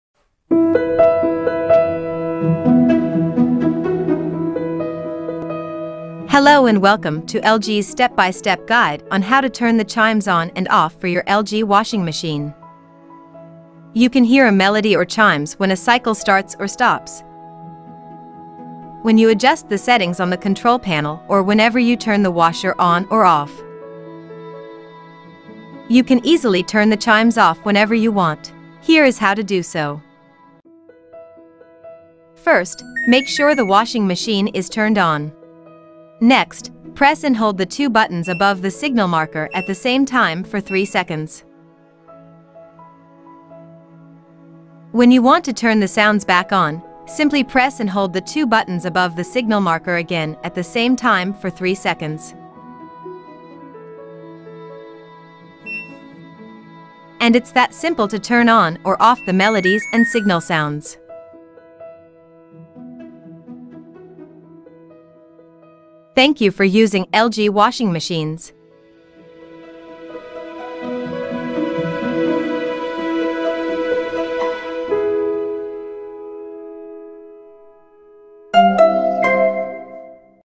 [Front load washer] Operation - Melody & Chimes
Hello and welcome to LG's step-by-step guide on how to turn the chimes on and off for your LG washing machine. You can hear a melody or chimes when a cycle starts or stops, when you adjust the settings on the control panel or whenever you turn the washer on or off. You can easily turn the chimes off whenever you want. Here is how to do so.